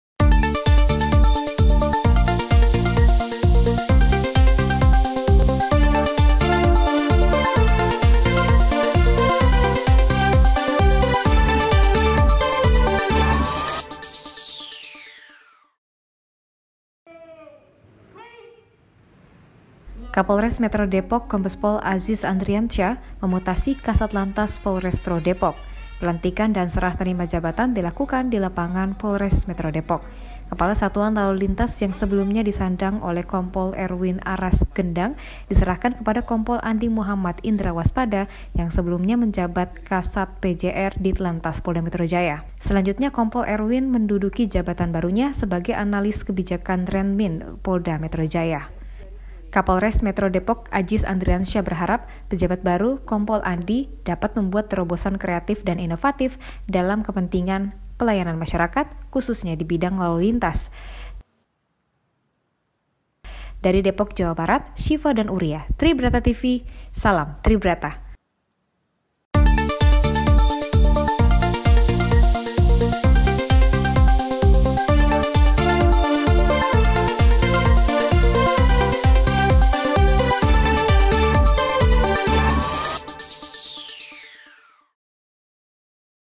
KAPOLRES METRO DEPOK MUTASI KASATLANTAS LAMA DAN MENGGANTI KASATLANTAS YANG BARU
Kapolres Metro Depok Kombes Azis Andriansyah memutasi Kasat Lantas Polrestro Depok. Pelantikan dan serah terima jabatan dilakukan di lapangan Polres Metro Depok.

Kepala Satuan Lalulintas yang sebelumnya disandang Kompol Erwin Aras Genda diserahkan kepada Kompol Andi Muhammad Indra Waspada yang sebelumnya menjabat kasat PJR Ditlantas Polda Metro Jaya. Selanjutnya Kompol Erwin menduduki jabatan barunya sebagai Analis Kebijakan Renmin Polda Metro Jaya.

Kapolres Azis Andriansyah berharap pejabat baru Kompol Andi dapat membuat terobosan kreatif dan inovatif dalam kepentingan pelayanan masyarakat khususnya dibidang lalu lintas.

Tim Liputan : Syifa / Uriah

Salam Tribrata.